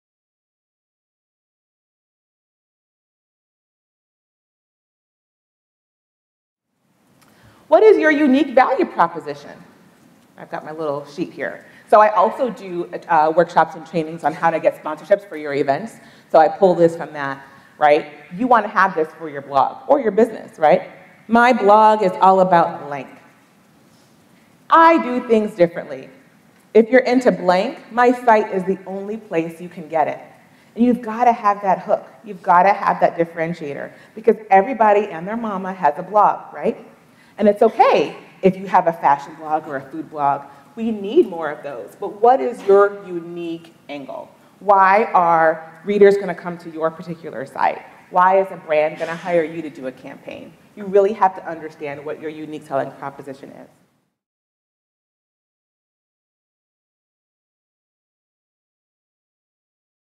Y'all Connect - Your blog's unique selling proposition
★ Stacey Ferguson explains why your blog must stand out among competitors. This clip is from her 2015 talk at yallconnect, June 12, 2015, in Birmingham, Alabama.